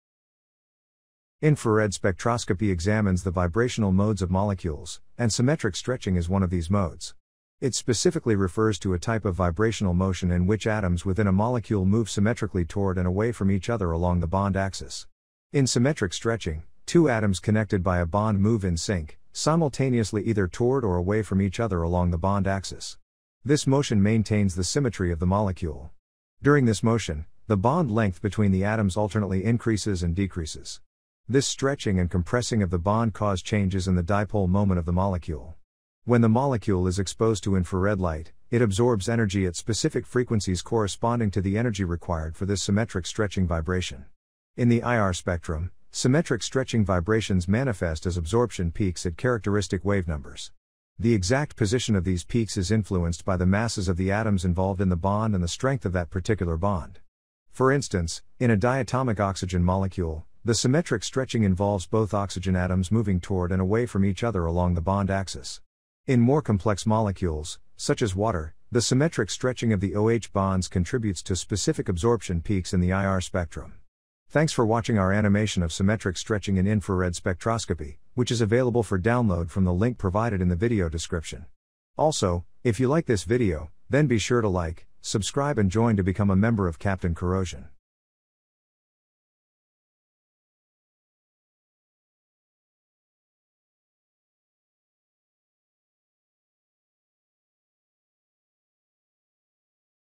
Symmetric Stretching in Infrared Spectroscopy Animation
Animation of symmetric stretching in infrared (IR) spectroscopy.